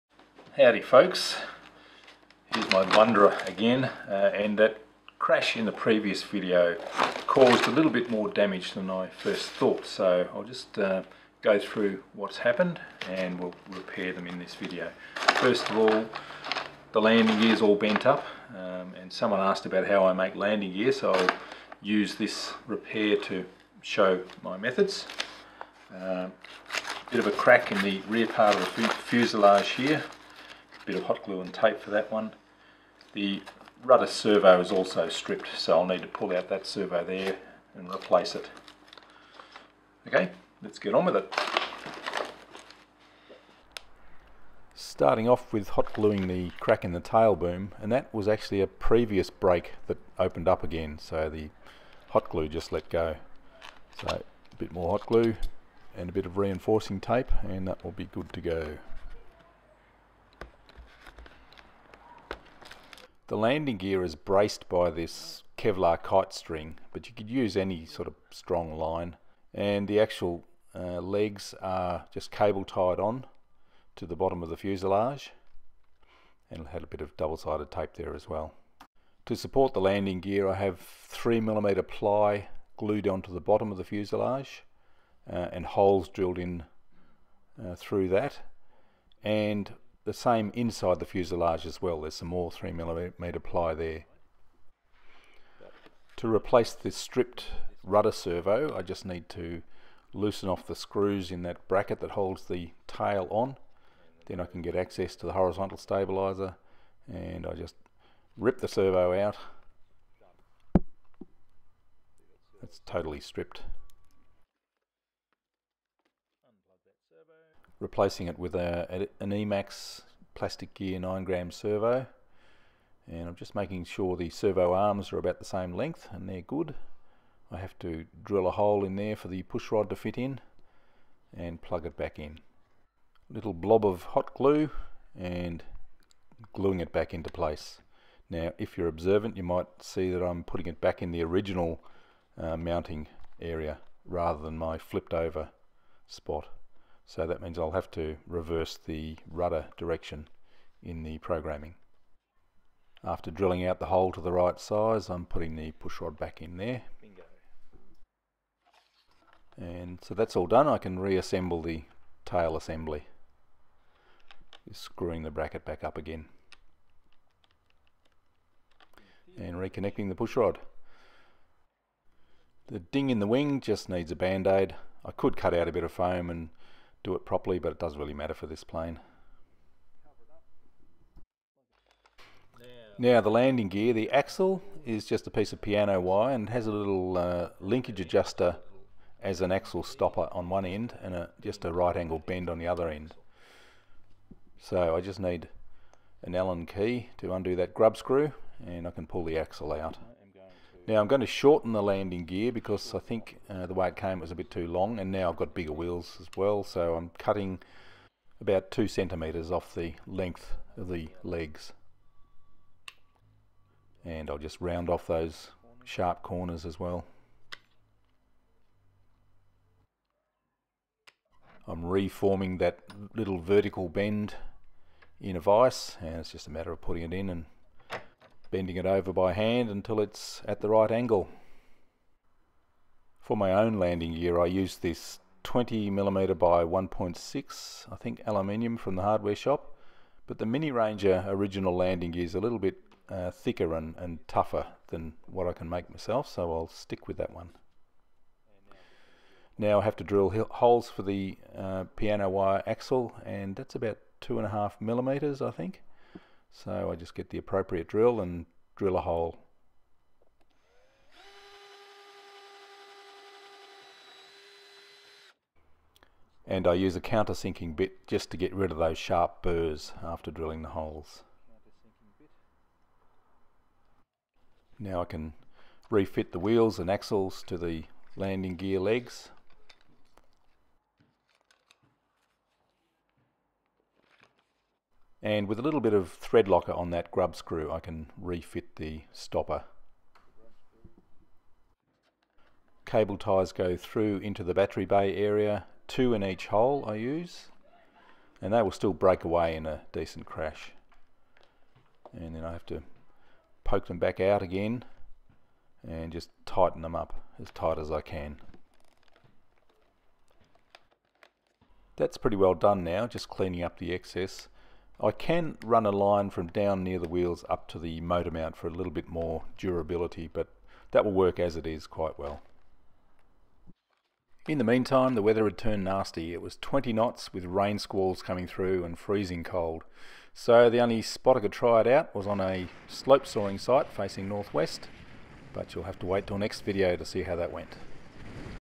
Vundra repair and landing gear
The crash in the previous video caused more damage than I first thought. Here are the repairs and also how to build landing gear.